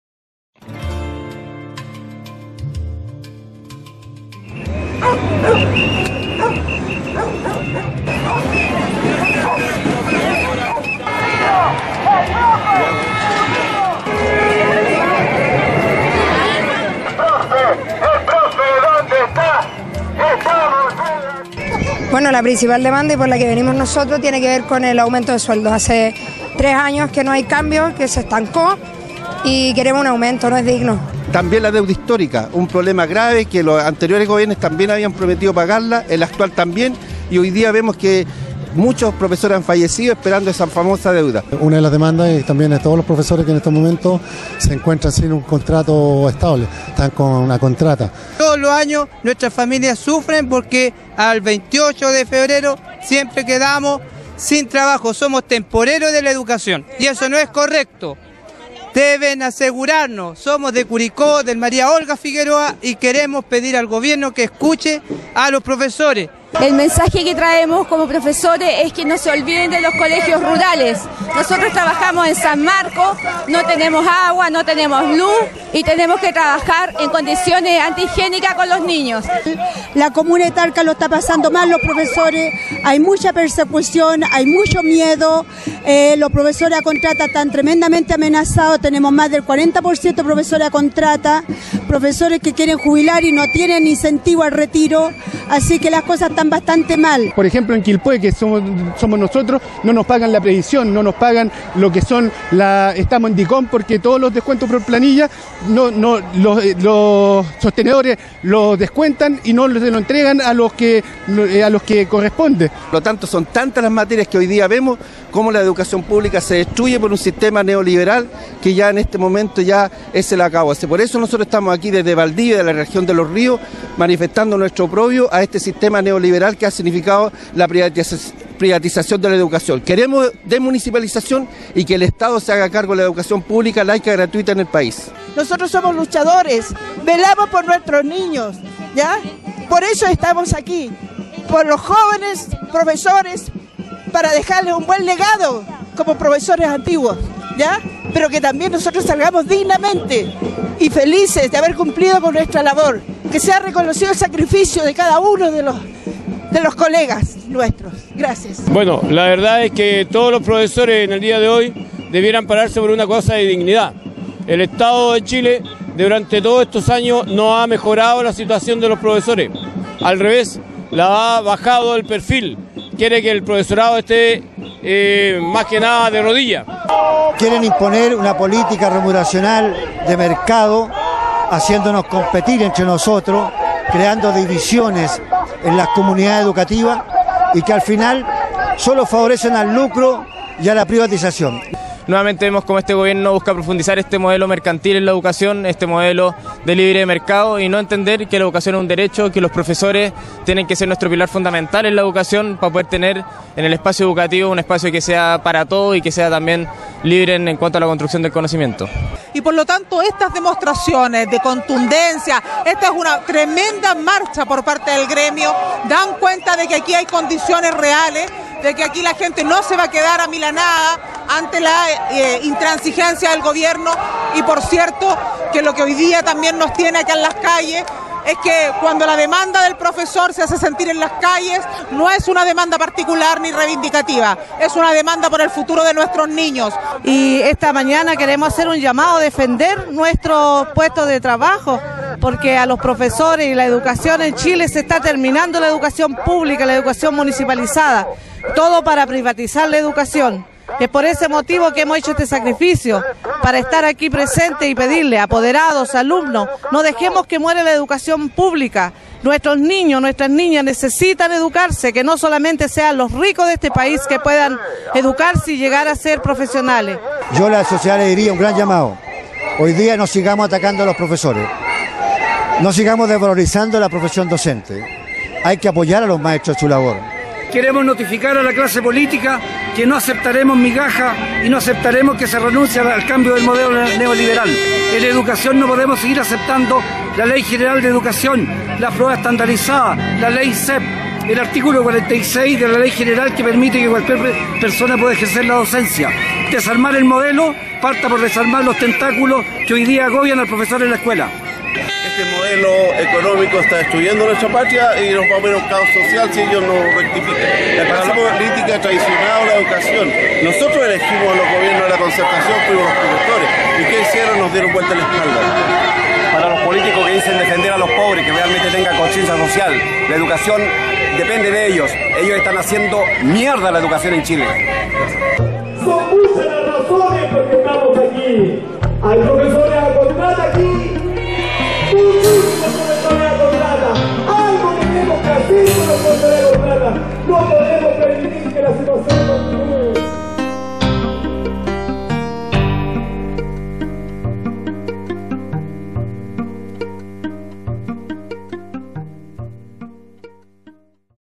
CHILE: PROFESORES EN JORNADA DE PARO NACIONAL
ESTO NO SALDRA EN TV, COMPARTE, COMENTA, DIFUNDE,
Educadores chilenos se manifiestan por la crisis de la Educaciòn estatal. Miles de profesores de distintas regiones de Chile y de la Región Metropolitana, se reunieron durante esta mañana en la Plaza Italia de Santiago, para participar en la convocatoria de Marcha en el marco del Paro Nacional por las demandas del magisterio.
Entre las principales demandas están:
1-. Fin a la contratación de profesores a plazo fijo de Marzo - Diciembre
2-. Plan de retiro voluntario con indemnización a los Profesores a jubilarse
3-. Piso mínimo sueldo Docente sin discriminación de sostenedor ( público, particular subvencionado, particular)
4-.Pago de la deuda histórica que se le adeuda a los profesores desde el traspaso de las escuelas del Ministerio a las Municipalidades.
5-.Carrera profesional Docente Permanente.
6-. Desmunicipalización de la educación, que las escuelas vuelvan al estado.
La movilizaciòn culminò a excasos metros del palacio del Gobierno chileno, con un acto cultural y discursos de los dirigentes colegiados.